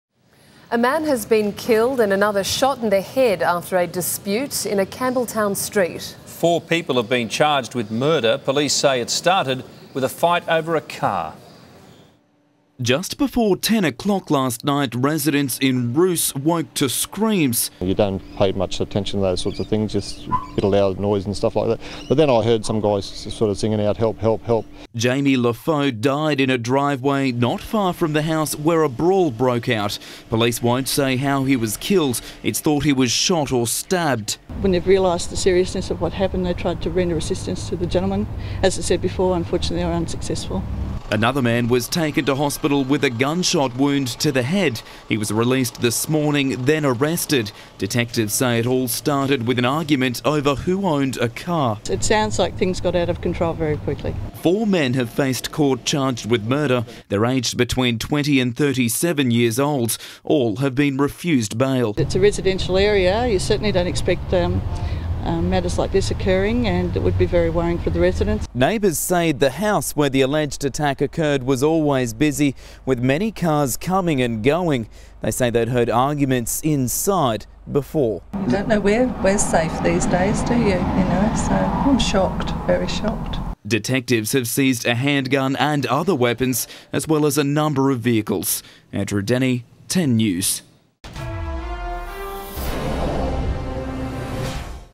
Fatal Shooting
A man has been shot dead and another critically wounded during a dispute over a car.